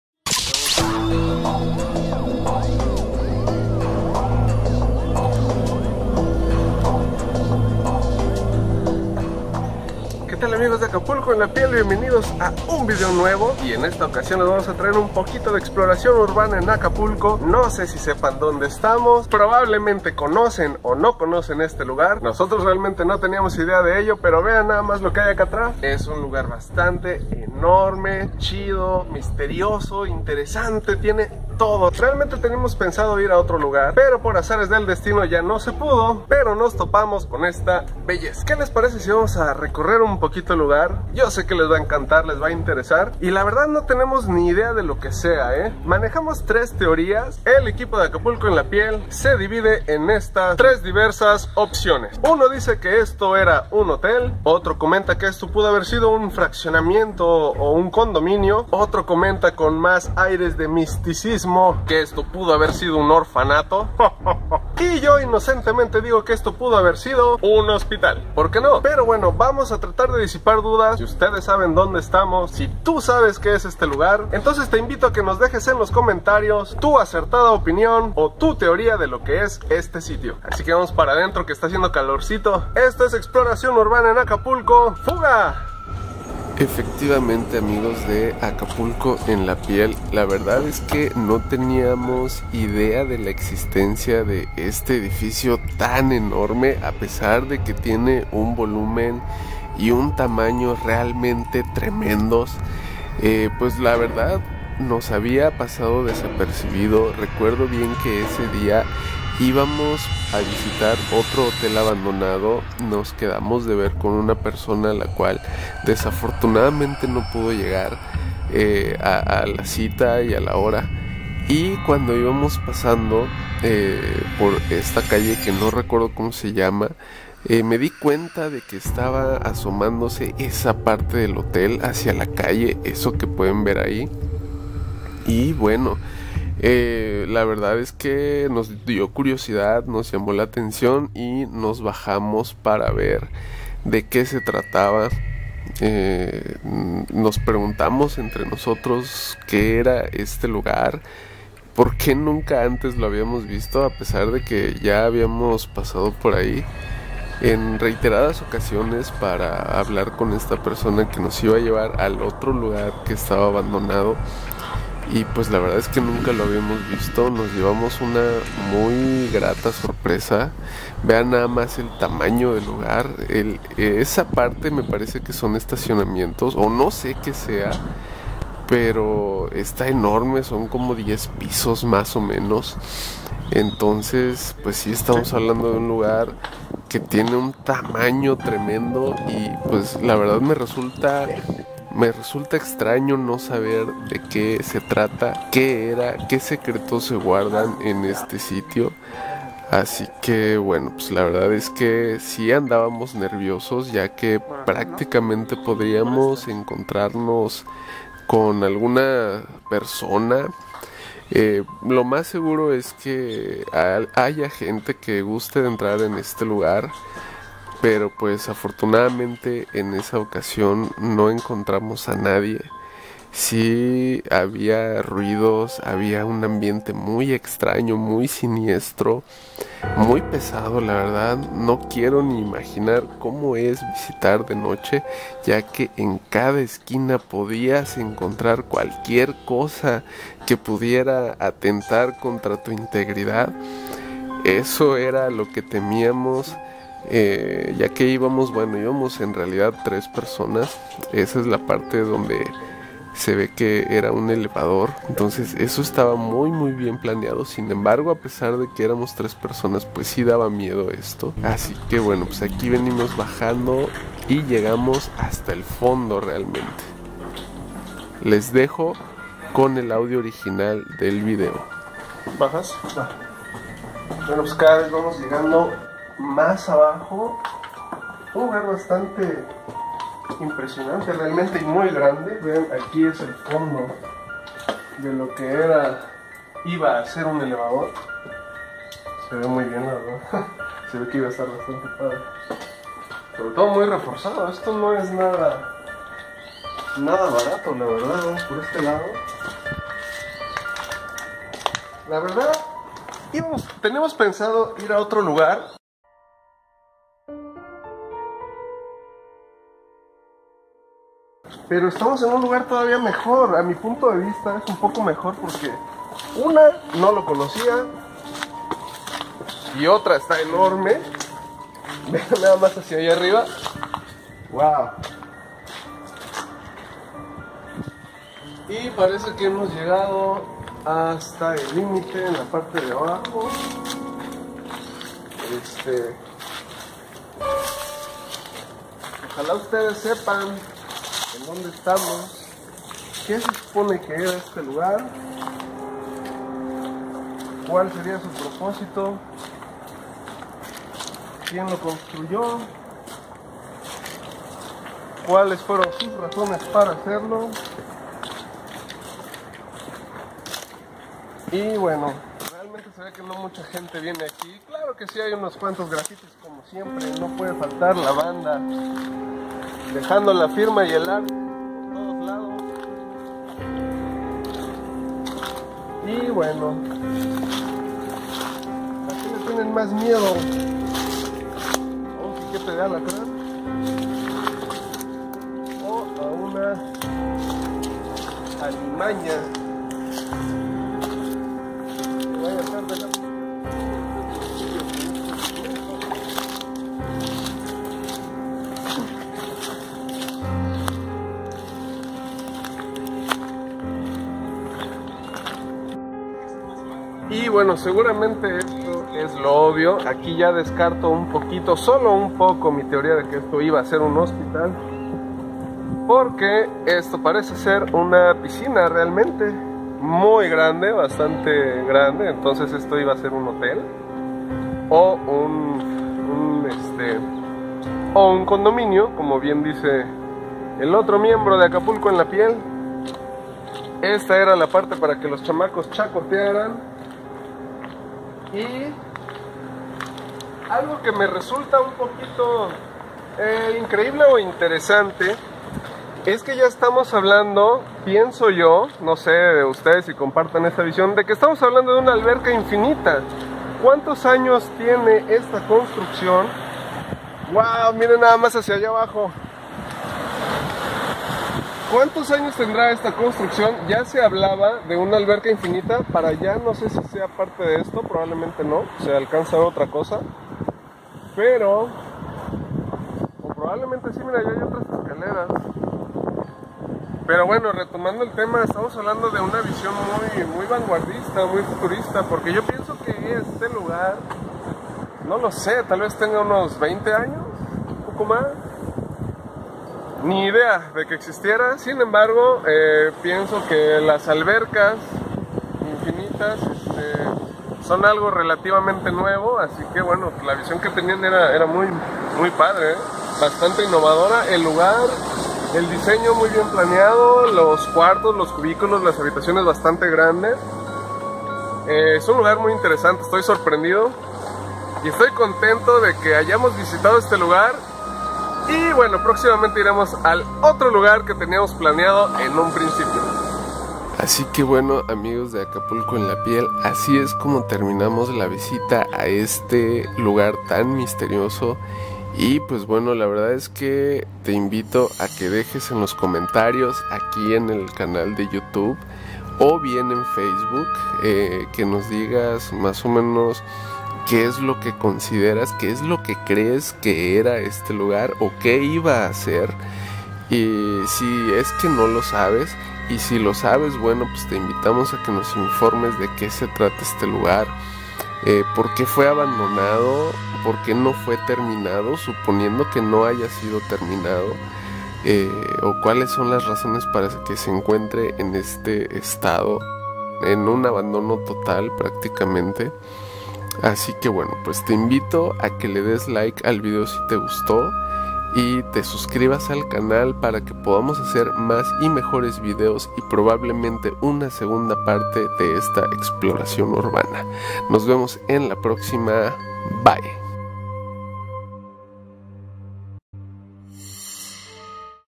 SOMOS LOS PRIMEROS EN ENTRAR A ESTE HOTEL ABANDONADO EN ACAPULCO //
FACEBOOK: ACAPULCO EN LA PIEL

ACAPULCO
EXPLORACION URBANA
HOTEL ABANDONADO EN ACAPULCO
HOTEL PIEDRA DEL ELEFANTE